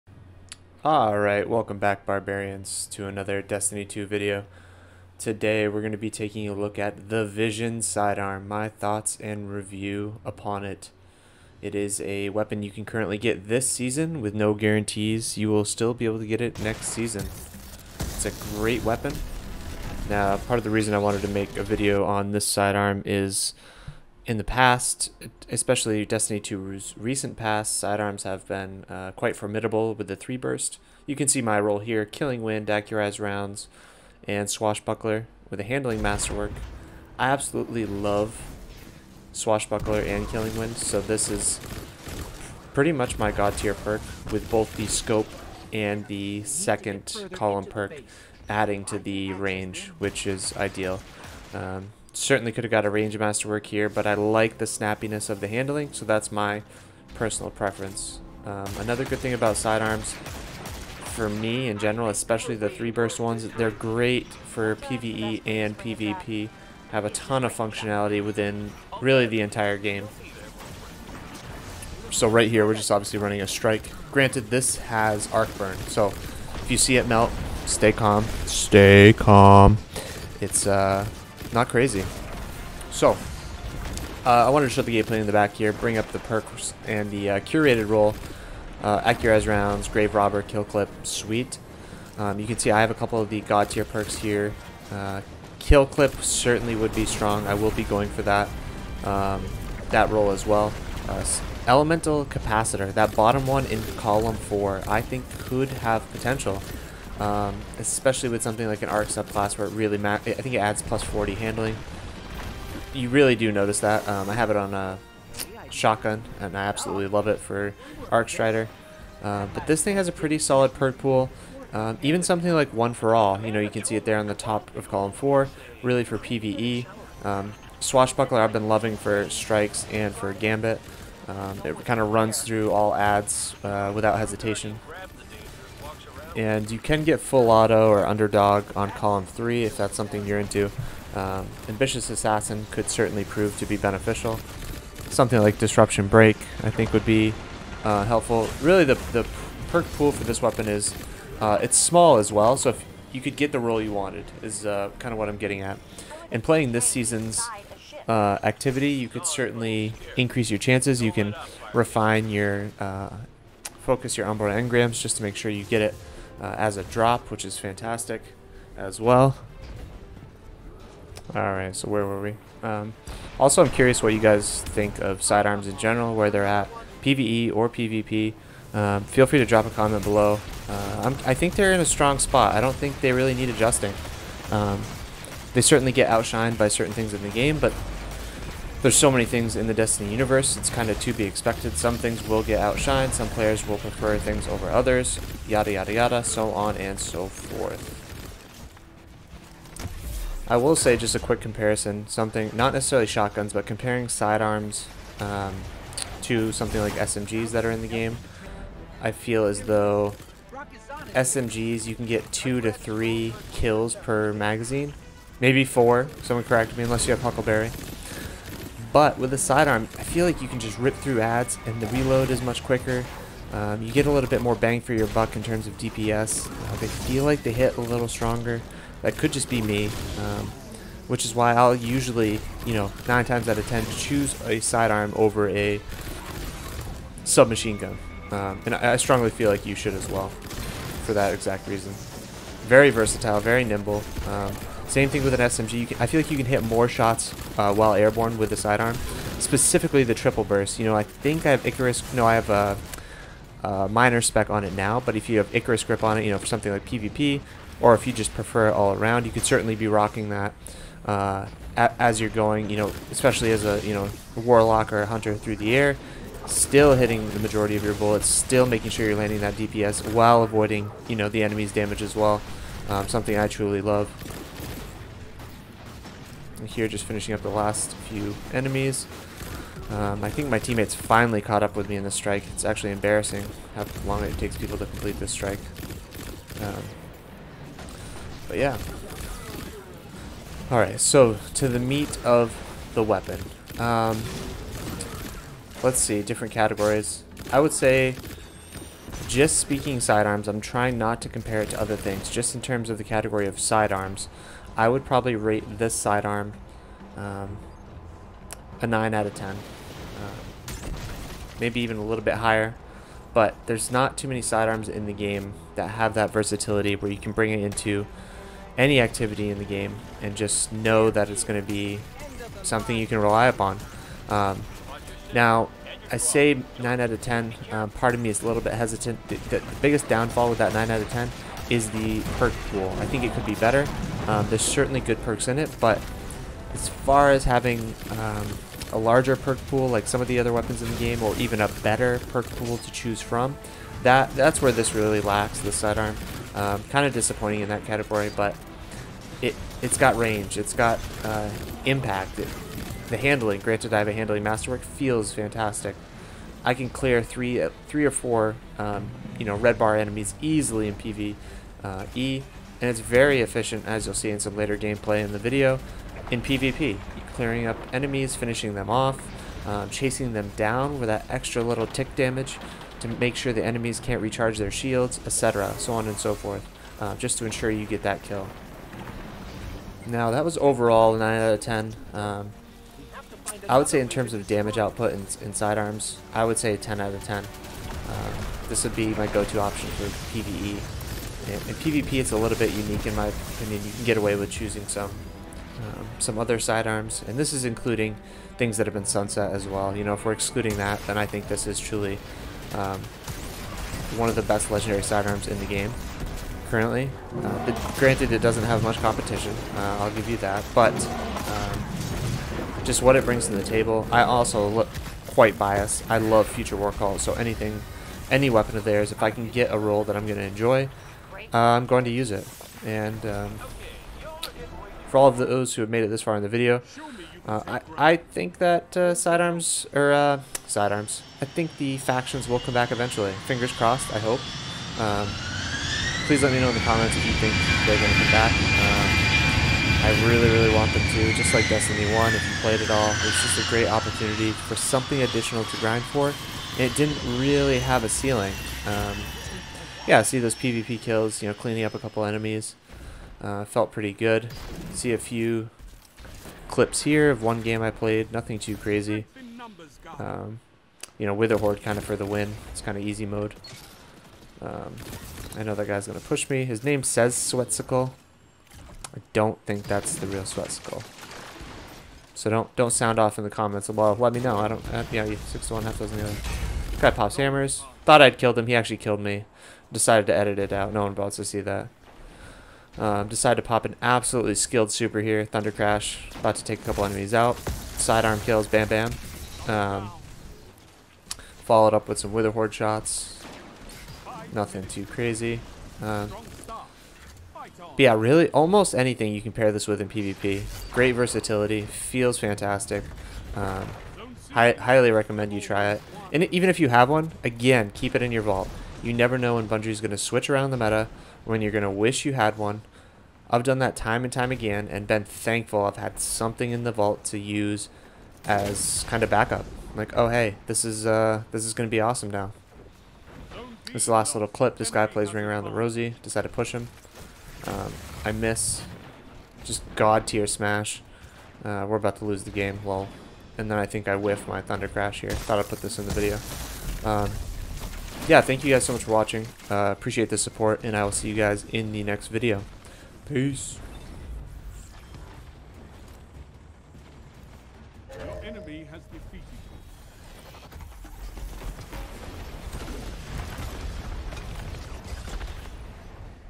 Best Sidearm Currently (according to me)
Let me know if you have a legendary sidearm that you enjoy more or even a exotic one that you use.

I personally love Future War Cult so anything related to that faction automatically feels great to me (totally bias here)